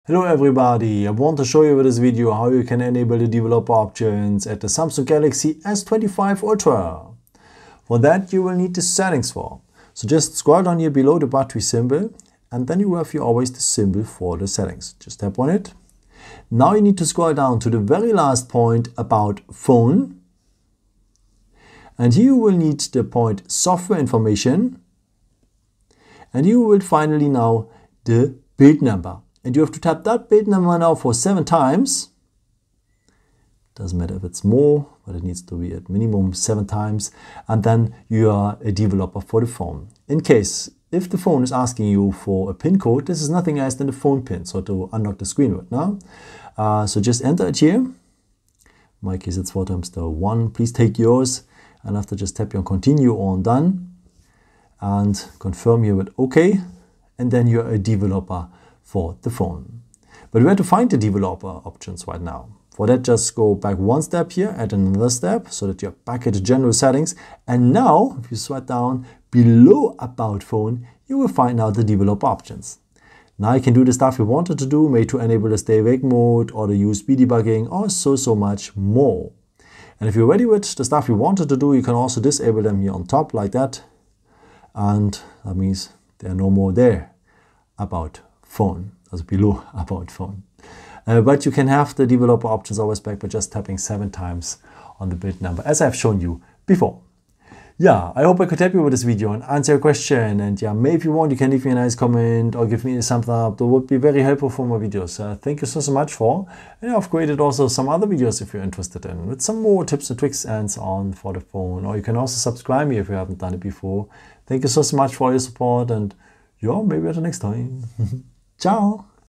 Samsung Galaxy S25 Ultra: How to enable the Developer Options? for USB Debugging etc.
With this video, I want to show you, how you can activate the developer options at the Samsung Galaxy S25 Ultra 5G.

 I will show you also, how you can disable the developer options at your Samsung S25 Ultra after.